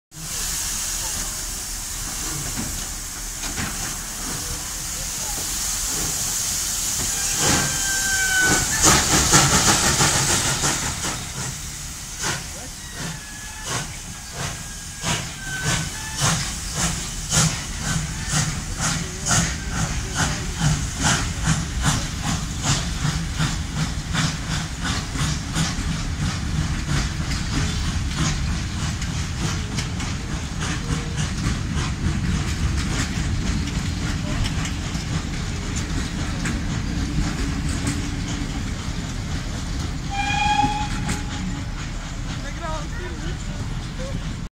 Ol49-69 Parowóz z pociągiem retro rusza ze stacji Parada parowozów Wolsztyn 2018
Parada Parowozów Wolsztyn 2018r.
Parada w Wolsztynie. Parowóz Ol49-69 przemianowany z 99 rusza ze składem zabytkowych wagonów PKP.